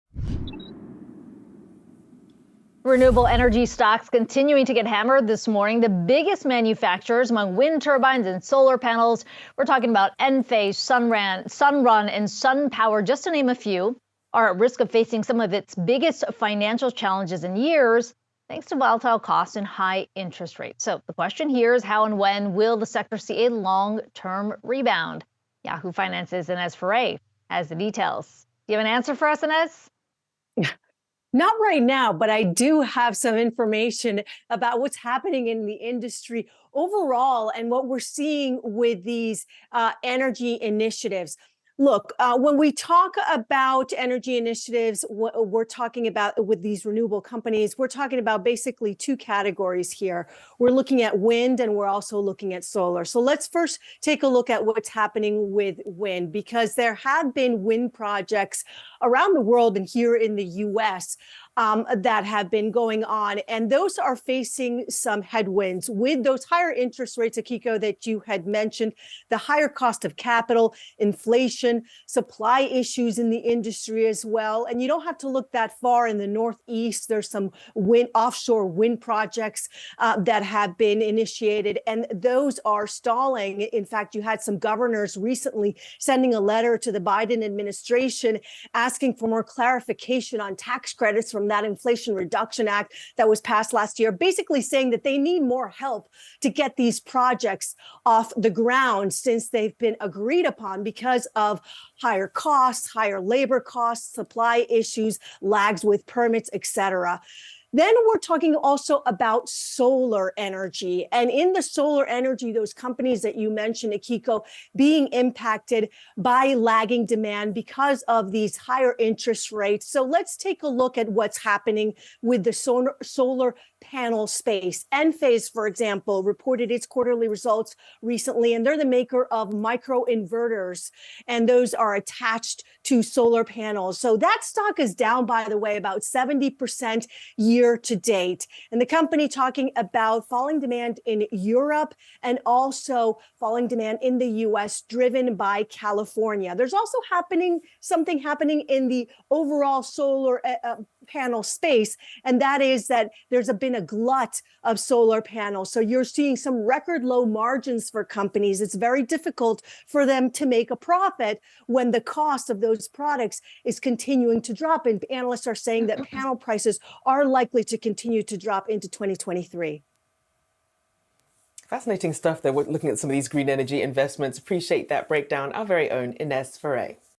Renewable energy stocks face pressure despite surge in installations
Shares of wind and solar companies continue their slide lower as interest rates stay higher for longer and rising costs stall new projects. Yahoo Finance’s Ines Ferre reports on oversupply and how this could impact energy stocks into 2024.